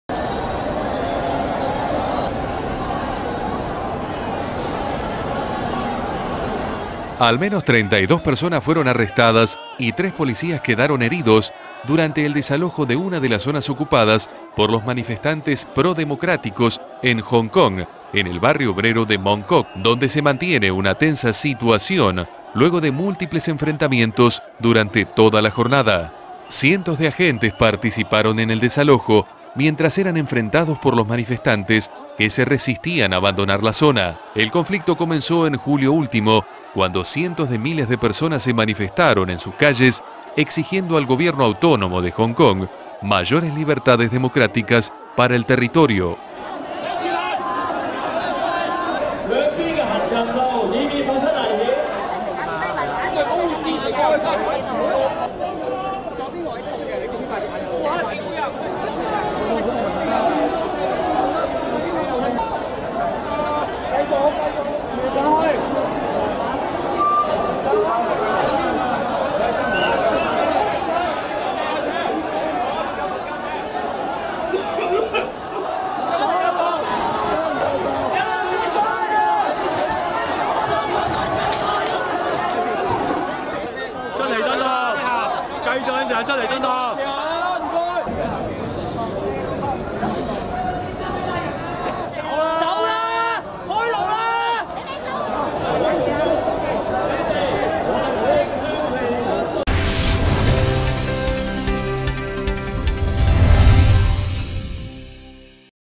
Al menos 32 personas presas y tres policías heridos por desalojo de manifestantes en Hong Kong
Al menos 32 personas fueron arrestadas y tres policías quedaron heridos hoy durante el desalojo de una de las zonas ocupadas por los manifestantes pro democracia en Hong Kong, en el barrio obrero de Mong Kok, donde se mantiene una tensa situación luego de múltiples enfrentamientos durante toda la jornada.

Cientos de agentes participaron en el desalojo de un tramo de cien metros en la calle Arglye, mientras eran enfrentados por los manifestantes que se resistían a abandonar la zona, dando lugar a confrontaciones que se prolongan ya durante más de nueve horas, informó la agencia de noticias EFE.

Tras lanzar una última advertencia a los manifestantes que se resistieron a abandonar la calle en la que las autoridades trataban de ejecutar una orden judicial de desalojo, la Policía provista de escudos y cascos empujó a los manifestantes fuera de las zonas ocupadas mientras muchos de los concentrados se resistían.

La llegada de agentes antidisturbios, que se sumaron a los cientos de policías en una nueva operación en la que llevaron a cabo detenciones acusando a los concentrados de participar en "reuniones ilegales", colaboró para aumentar la tensión, que en las primeras horas de la noche mantiene en vilo a la zona.

La operación comenzó a primera hora de esta mañana y en un primer momento, los manifestantes no opusieron resistencia física a la operación pero sí profirieron consignas de "queremos sufragio universal".

Yvvone Lleung, una de las líderes de la Federación de Estudiantes de Hong Kong, organización que hizo de interlocutora oficial con el Gobierno durante las protestas, retó a la Policía a que justificara su participación en el desalojo del área asignada bajo orden judicial.

La retirada de hoy obedece a una orden judicial del Tribunal Superior de Hong Kong, que autorizó la asistencia de la Policía para eliminar los obstáculos en una de las calles tomadas por los manifestantes en el populoso barrio, tras una demanda interpuesta por dos agrupaciones de taxistas y una empresa de autobuses, que alegaron que sus negocios estaban sufriendo por la interrupción del tráfico en la zona.

Se trata de la segunda intervención judicial llevada a cabo para desalojar a los manifestantes de las tres zonas de protestas -Admiralty, Causeway Bay y Mong Kok-, en las que permanecen desde hace casi dos meses.

El pasado 18 de noviembre, agentes judiciales procedieron a la eliminación de barricadas en un pequeña zona del área de Admiralty, donde se asienta el campamento principal de las protestas.

La operación de Mong Kok, considerada de alto riesgo, prevé desarrollarse a lo largo de toda la semana, con la eliminación de más barricadas instaladas en distintas calles del barrio.

Para mañana, la Policía tiene previsto desmantelar la zona ocupada de la calle Nathan, donde cientos de tiendas de campaña se extienden en un tramo de más de medio kilómetro de esta calle, una de las arterias de tráfico más importantes de la península de Kowloon.

El conflicto comenzó en julio último cuando cientos de miles de personas se manifestaron en sus calles exigiendo al gobierno autónomo de Hong Kong mayores libertades democráticas para el territorio, que durante un siglo fue colonia británica, y menos intervención del Gobierno de China en sus asuntos internos.

El 31 de agosto pasado China aceptó el principio de que el próximo líder de Hong Kong fuera elegido por sufragio universal en 2017, aunque limitó el número de posibles candidatos a dos o tres que deberían tener previamente el visto bueno de Beijing.

A partir de ese momento miles de manifestantes vestidos con ropas de color negro, especialmente estudiantes, recorrieron las calles de Hong Kong para pedir democracia y en protesta contra de la decisión del gobierno chino, que derivó en huelgas y ocupación de calles y avenidas del centro financiero de la ciudad, con carpas y vallas que entorpecieron el tránsito.